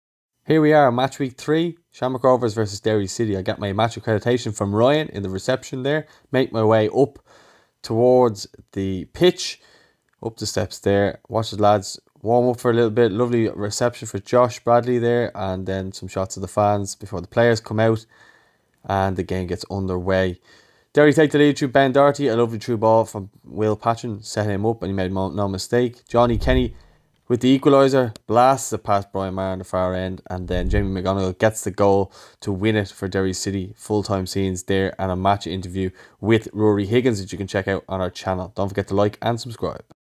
Shamrock Rovers 1-2 Derry City | Matchday Vlog
Shamrock Rovers 1-2 Derry City | Matchday Vlog. goals from Ben Doherty and Jamie McGonigle give Derry the away win at Tallaght stadium